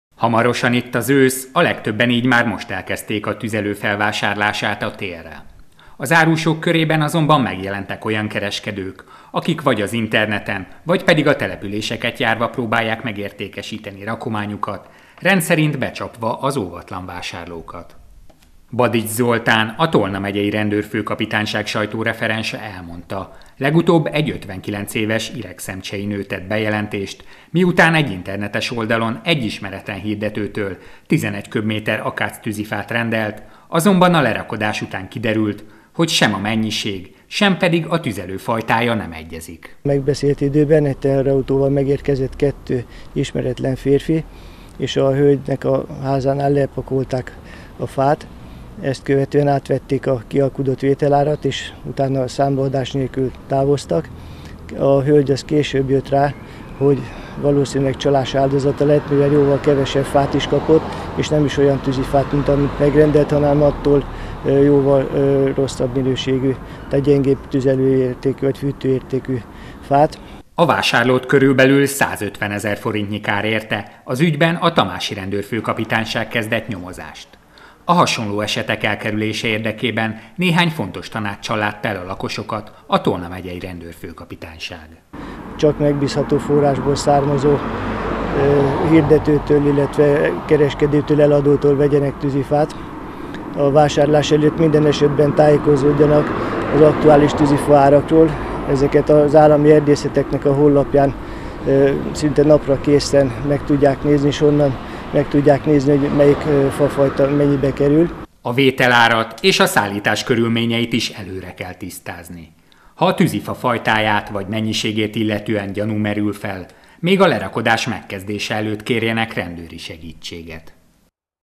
Vigyázat, tűzifacsalók
A tüzifa árak megemelkedésével párhuzamosan megjelentek az olyan értékesítők is, akik minél nagyobb haszonnal szeretnének üzletet csinálni. A csalók általában rosszabb minőségű, fűtőértékű fát kínálnak eladásra és rend szerint a mértékegység megjelölésével is trükköznek.